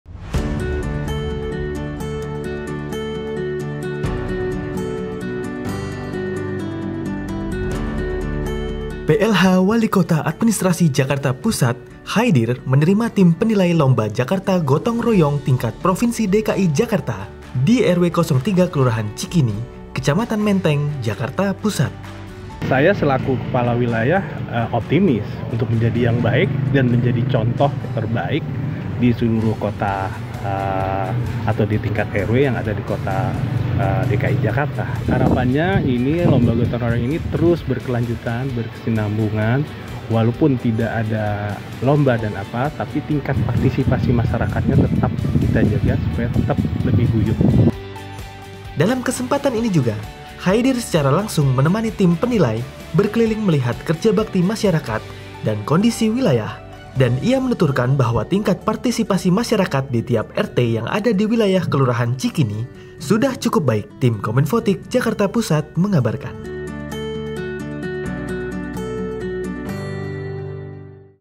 Plh Wali Kota Terima Tim Penilai Lomba Jakarta Gotong Royong Tingkat Provinsi
Plh Wali Kota Administrasi Jakarta Pusat Chaidir menerima Tim Penilai Lomba Jakarta Gotong Royong Tingkat Provinsi DKI Jakarta, di RW 03 Kelurahan Cikini, Kecamatan Menteng, Jakarta Pusat, Minggu (21/1).

Chaidir mengatakan, dirinya optimis Kelurahan Cikini bisa menjadi yang terbaik dalam Lomba Jakarta Gotong Royong. Terpilihnya Kelurahan Cikini menjadi Wakil Jakarta Pusat dalam lomba Jakarta Gotong Royong ini karena berdasarkan penilaian yang dilakukan sejak awal November 2023.

Dalam kesempatan ini, Chaidir secara langsung menemani tim penilai berkeliling melihat kerja bakti masyarakat dan kondisi wilayah menuturkan bahwa tingkat partisipasi masyarakat di tiap RT yang ada di wilayah Kelurahan Cikini sudah cukup baik.

Ia berharap, Kerja bakti gotong royong ini akan terus kita lakukan meskipun tidak ada lomba agar mempertahankan tingkat partisipasi masyarakat sehingga tetap lebih guyub.

Penulis Naskah : Maulana
Kameramen : Rendy Vhatra
Editor Video : Rendy Vhatra
Pengisi Suara : Dwi Arif S